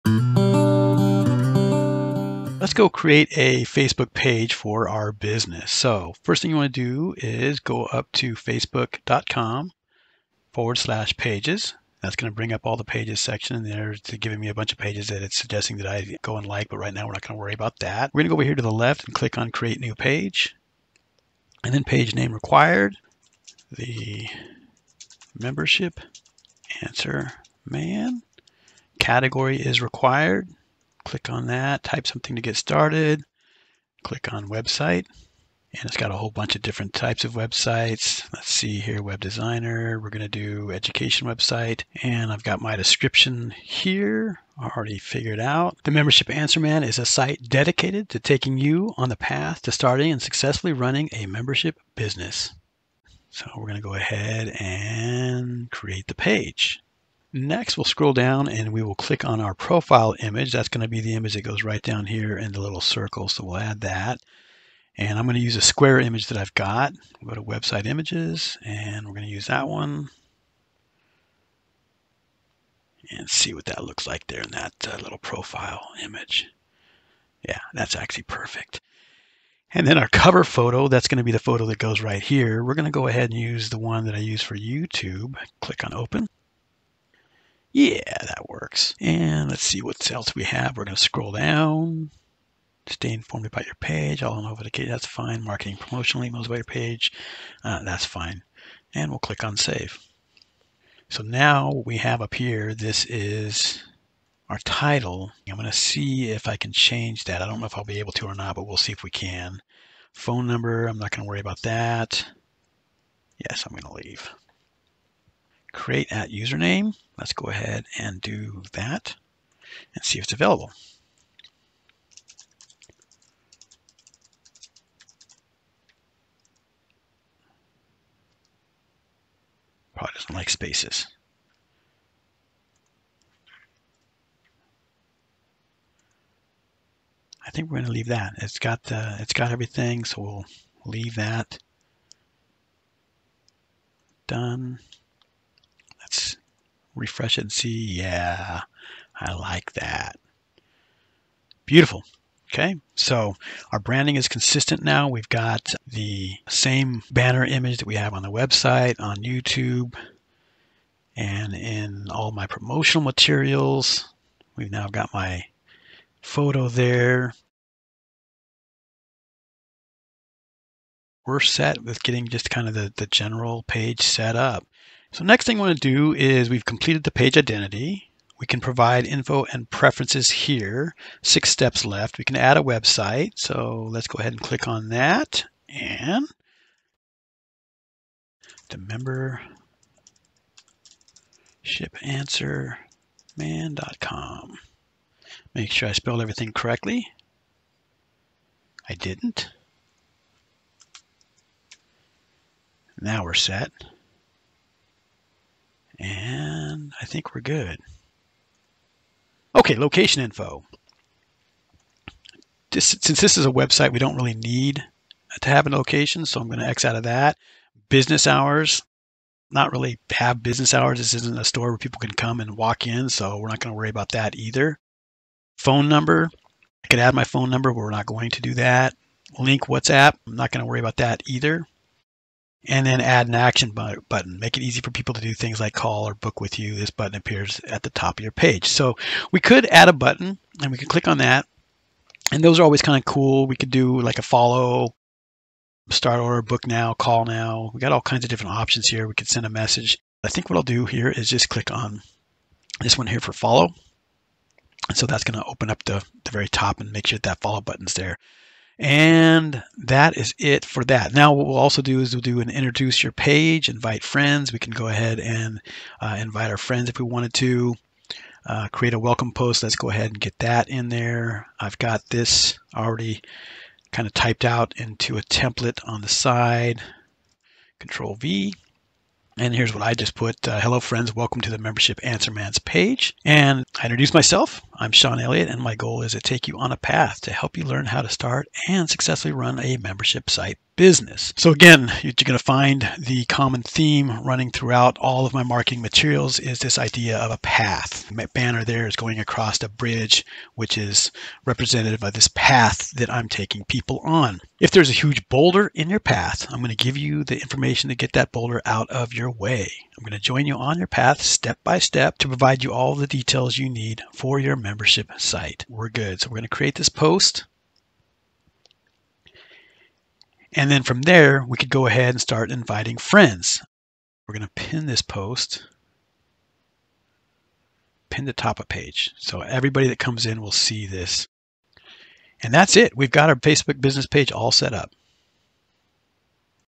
Setting Up Your Facebook Page
Are you wondering what you need to do to set up a Facebook Page for your business or project? In this video, I go over how to set up a page for your business on Facebook in less than 6 minutes.